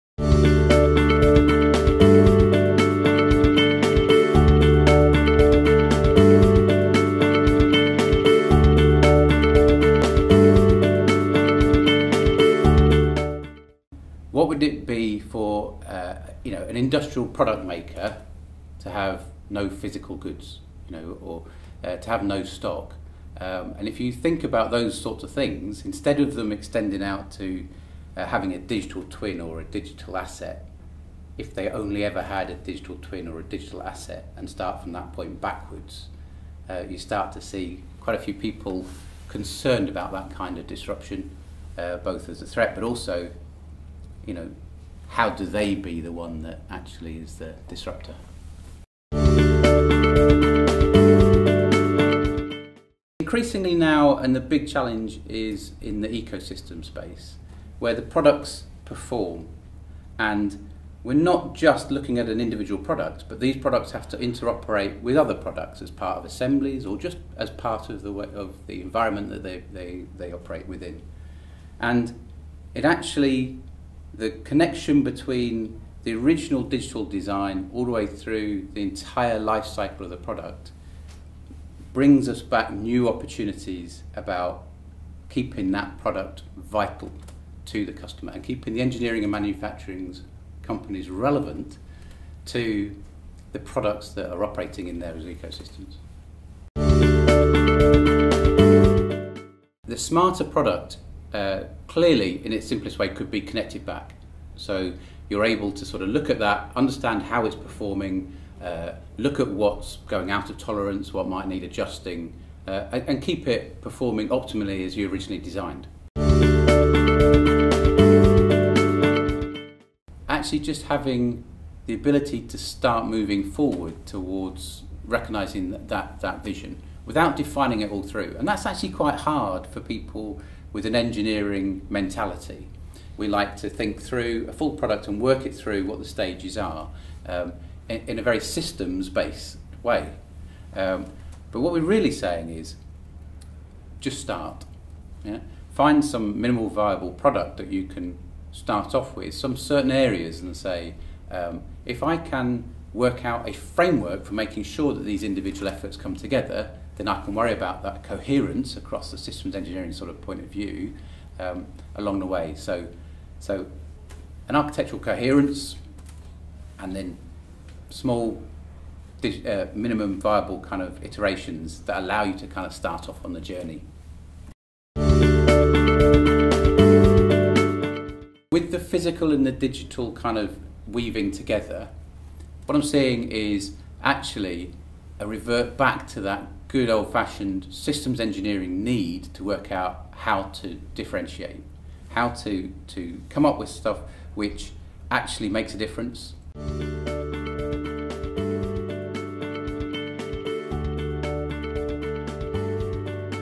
Engineering for Digital Transformation
The Manufacturer sat down with Paul Homan to discuss how digital disruption is changing manufacturing and  why bridging between the cyber and the physical world is so important.